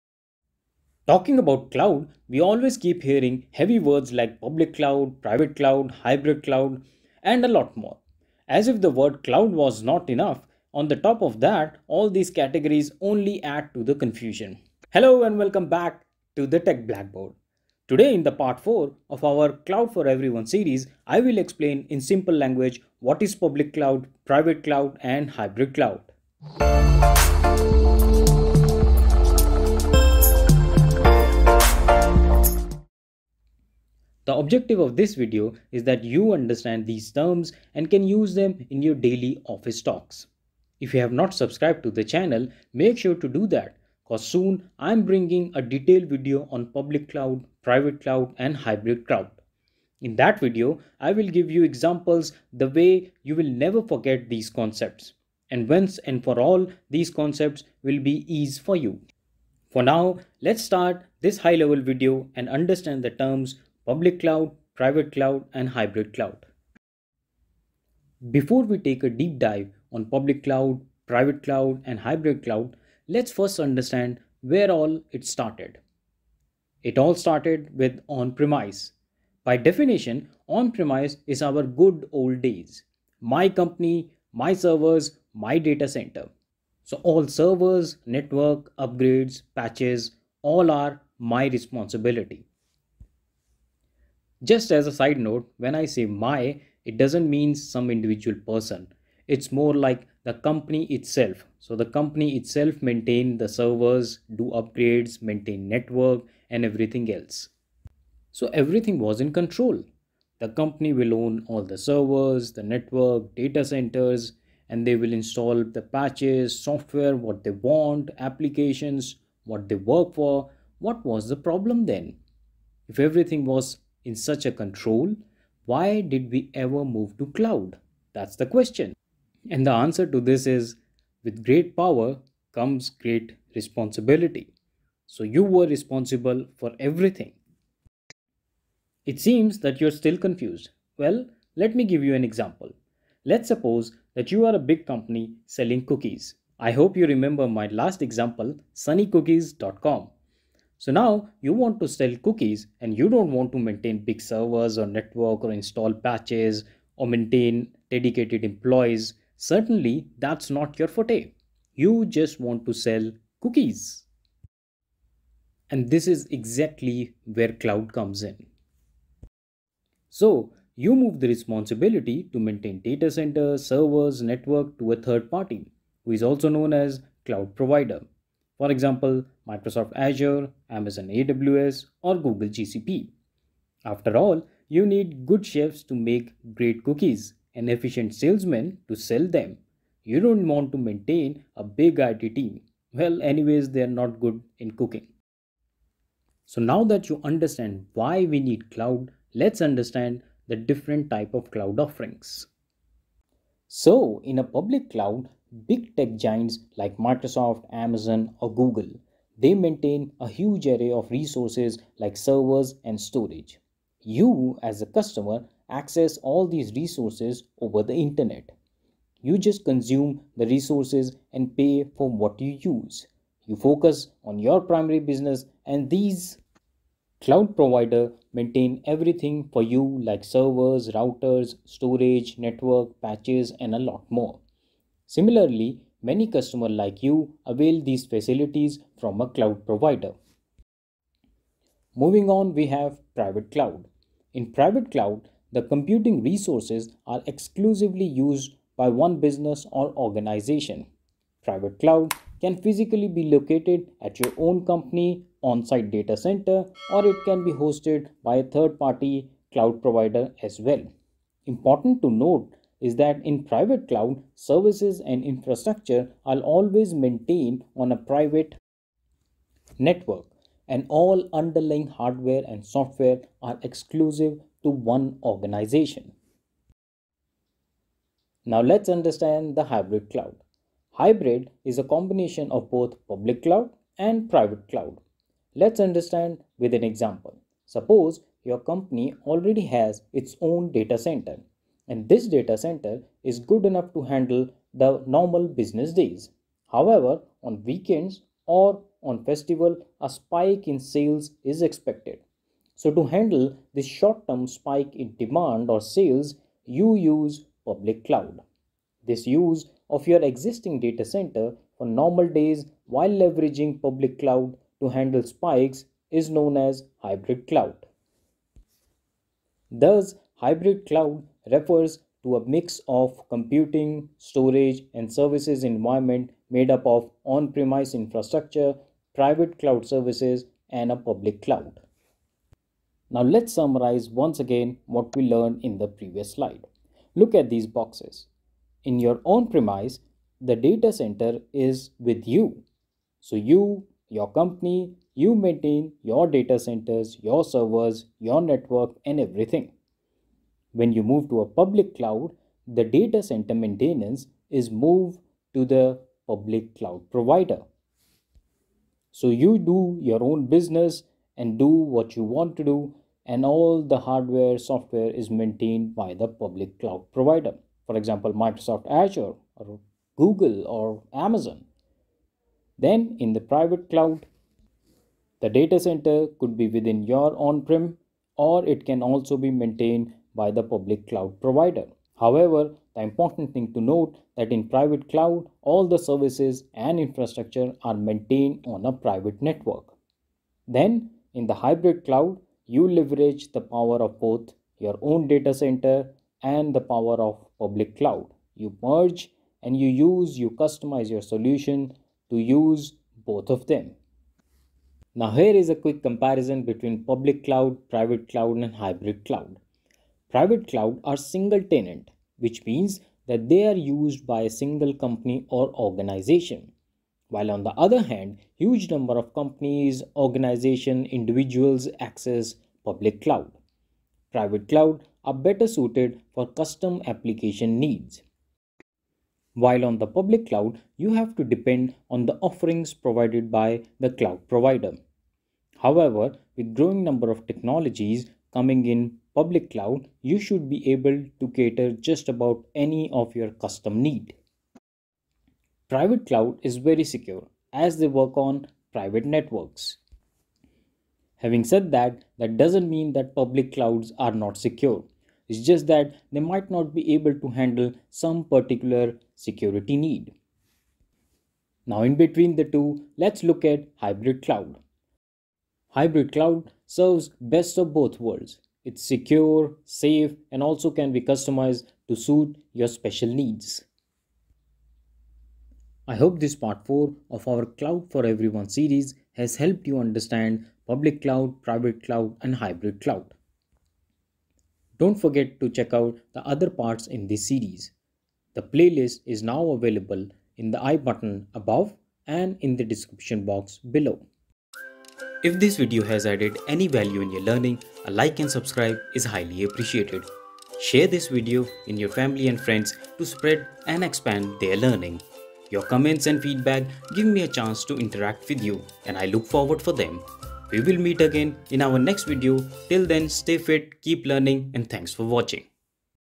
Azure Fundamental Part 4: High level Overview of Public vs Private vs Hybrid Cloud
Microsoft Azure Fundamentals: Full Course
Part 4: It Covers:
1. Public Cloud
2. Private Cloud
3. Hybrid Cloud

▬ Full Agenda of this video  ▬▬▬▬▬▬▬▬▬▬
00:00 Introduction
01: 11 Understanding On-Premises
03: 24 Understanding Public Cloud
04: 02 Understanding Private Cloud
04: 39 Understanding Hybrid Cloud
05: 56 Public Cloud vs Private Cloud vs Hybrid Cloud